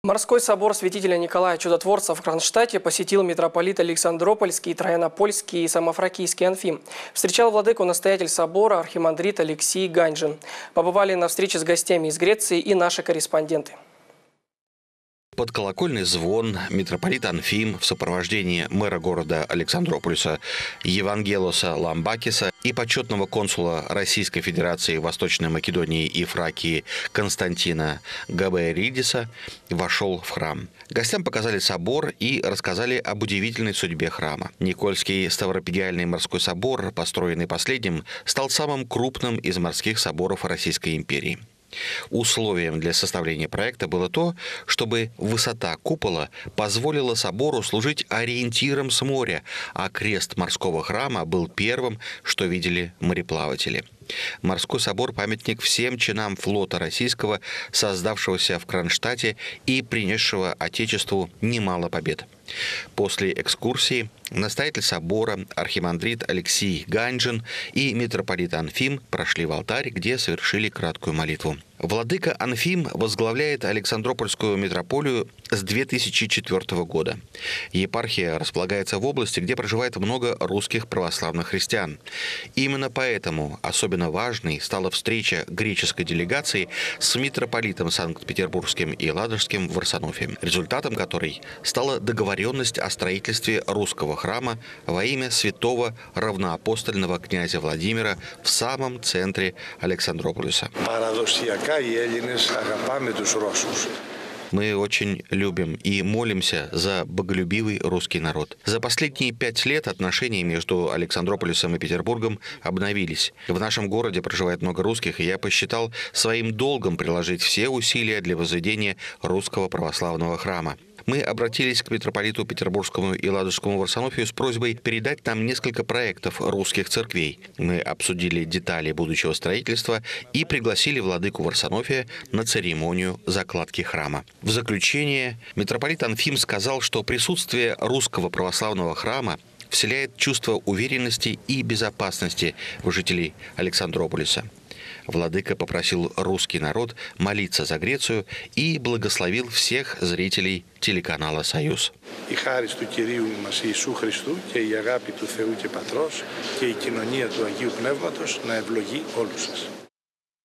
Морской собор Кронштадта посетили гости из Греции
Морской собор святителя Николая Чудотворца в Кронштадте посетил митрополит Александропольский, Траянопольский и Самофракийский Анфим. Встречал владыку настоятель собора архимандрит Алексий Ганьжин. Побывали на встрече с гостями из Греции и наши корреспонденты.